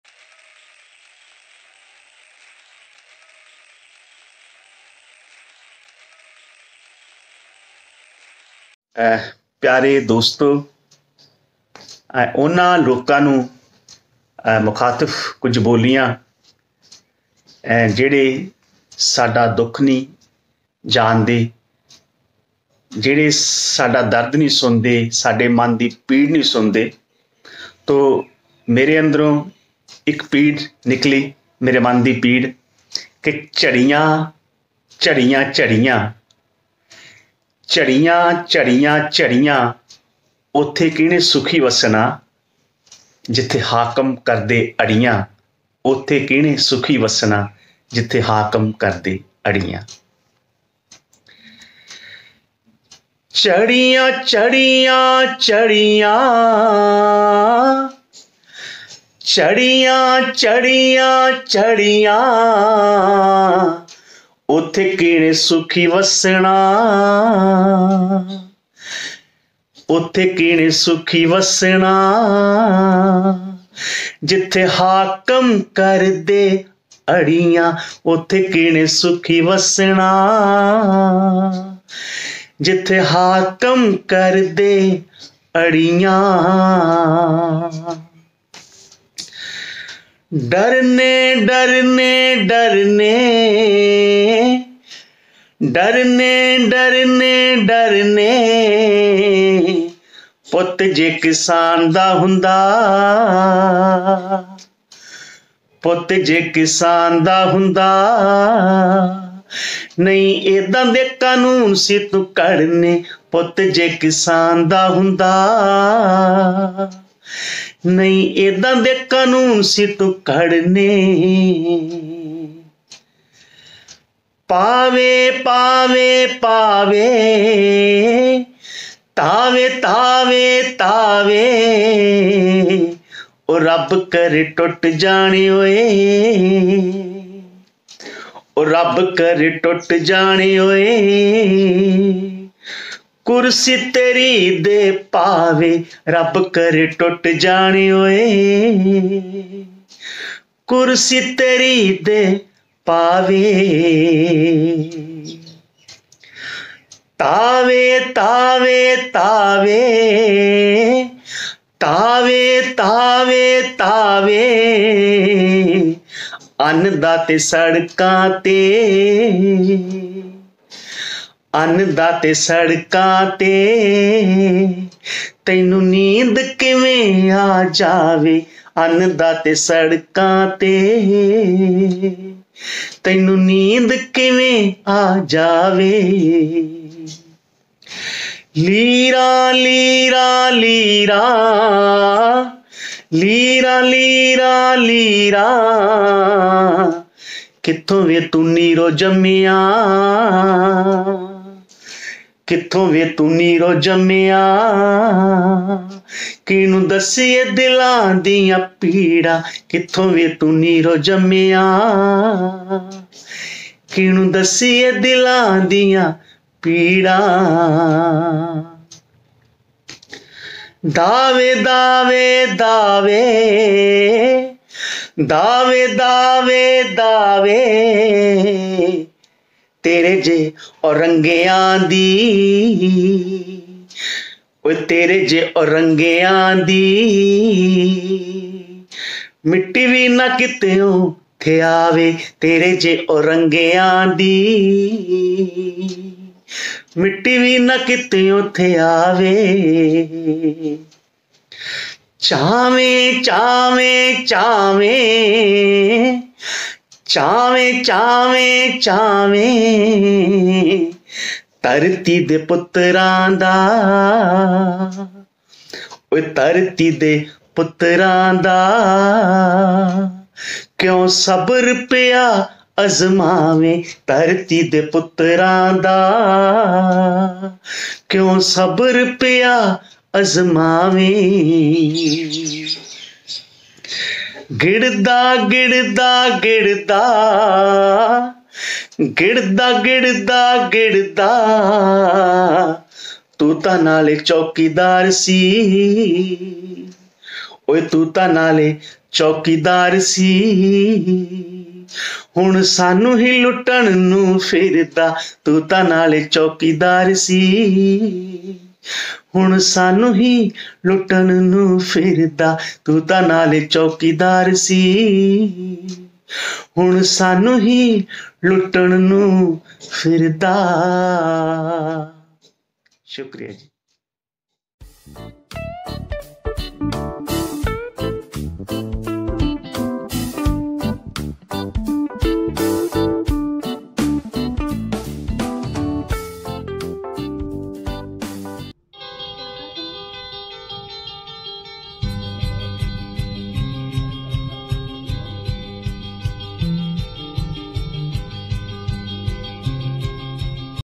Boliyan|| پنجابی بولیاں || Tarlochan Lochi || khayal TV || ਤਰਲੋਚਨ ਲੋਚੀ
چڑھدے پنجاب توں کساناں دے احتجاج وچ شامل ودھیا شاعر ترلوچن لوچی دیاں پنجابی بولیاں ،،،،،
|| خیال ٹی وی ||سبسکرائب کیجئے پلیز ||باذوق احباب کا اپنا یو ٹیوب ادبی چینل
If you like the videos SHARE MAXIMUM.
Regards: Anjum Saleemi